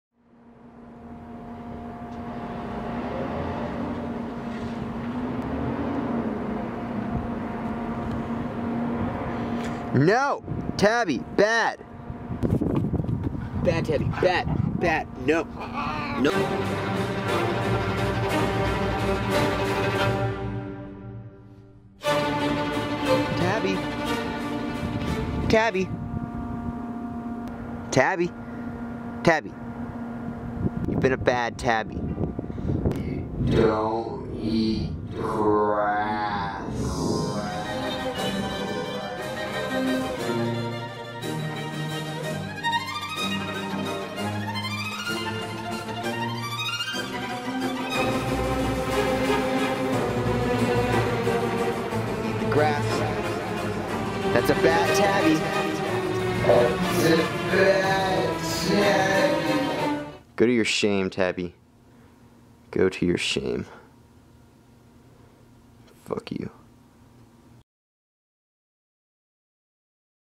Bad Tabby
Tabby eats grass but she's not supposed to eat grass. Tabby's bad, she's a bad tabby.